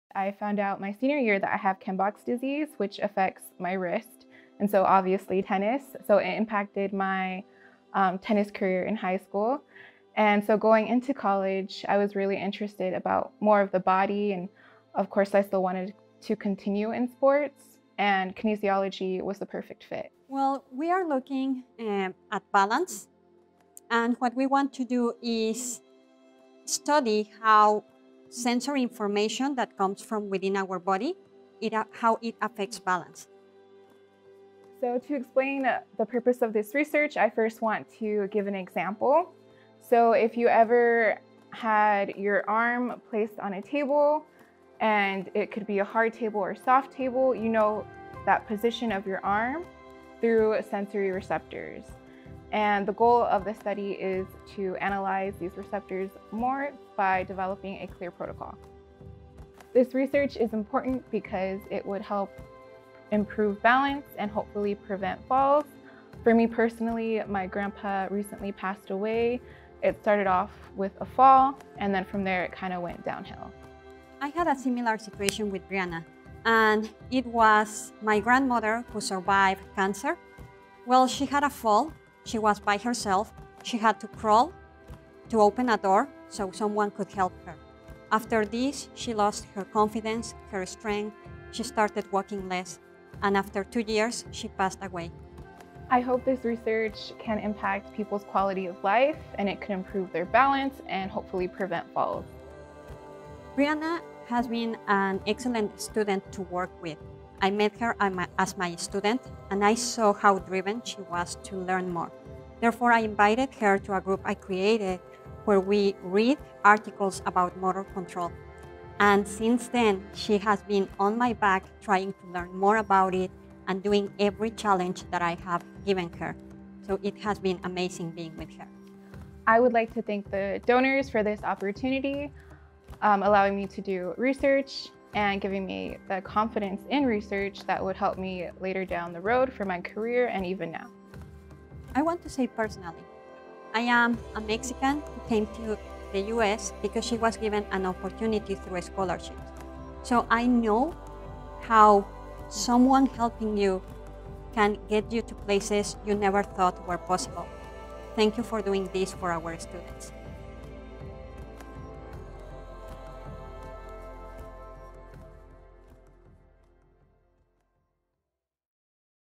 Student Research Scholar: Brianna Maldonado
The President’s Associates is the most prestigious and longest-standing giving society at California State University, Bakersfield. The President’s Associates supports scholarships to conduct in-depth research projects as part of the Student Research Scholars Program. This program gives students like Brianna Maldonado hands-on experience with faculty mentors.